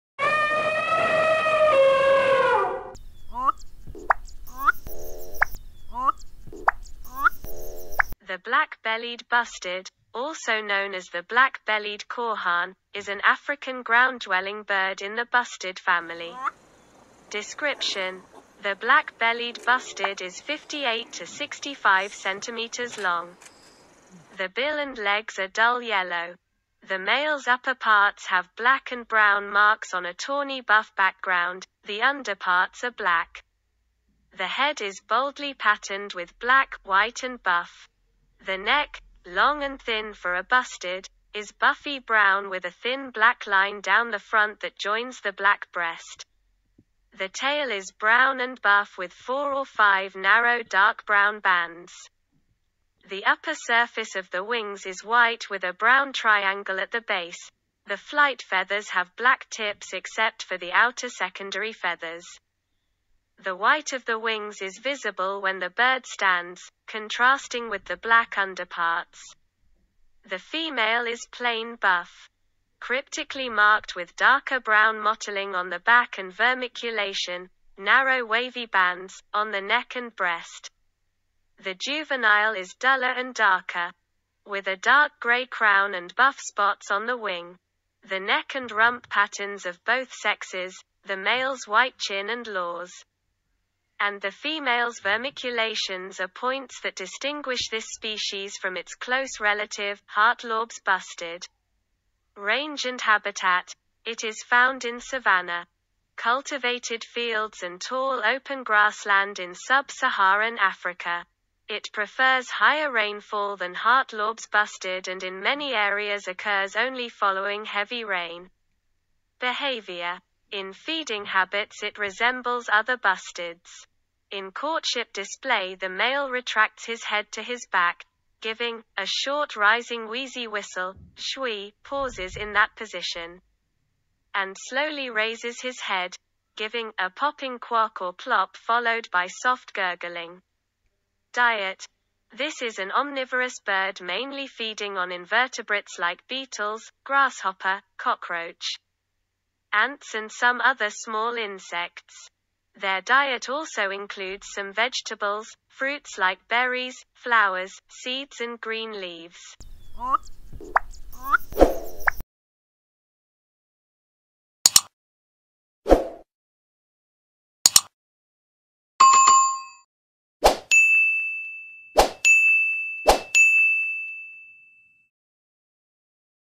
The black bellied bustard || Bustard Chapter - 6 || The Rup
The black-bellied bustard , also known as the black-bellied korhaan, is an African ground-dwelling bird in the bustard family.
Description :
The black-bellied bustard is 58–65 cm long. The bill and legs are dull yellow. The male's upperparts have black and brown marks on a tawny buff background ,  the underparts are black. The head is boldly patterned with black, white and buff. The neck, long and thin for a bustard, is buffy brown with a thin black line down the front that joins the black breast. The tail is brown and buff with four or five narrow dark brown bands. The upper surface of the wings is white with a brown triangle at the base; the flight feathers have black tips except for the outer secondary feathers. The white of the wings is visible when the bird stands, contrasting with the black underparts.
The female is plain buff, cryptically marked with darker brown mottling on the back and vermiculation (narrow wavy bands) on the neck and breast. The juvenile is duller and darker, with a dark grey crown and buff spots on the wing. The neck and rump patterns of both sexes, the male's white chin and lores, and the female's vermiculations are points that distinguish this species from its close relative, Hartlaub's bustard.
Range and habitat
It is found in savanna, cultivated fields and tall open grassland in sub-Saharan Africa . It prefers higher rainfall than Hartlaub's bustard and in many areas occurs only following heavy rain.
Behaviour :
In feeding habits it resembles other bustards. In courtship display the male retracts his head to his back, giving "a short rising wheezy whistle, zhweeeeee", pauses in that position, and slowly raises his head, giving "a popping quock or plop followed by soft gurgling".
Diet :
This is an omnivorous bird mainly feeding on invertebrates like beetles, grasshopper, cockroach, ants and some other small insects. Their diet also includes some vegetables, fruits like berries, flowers, seeds and green leaves.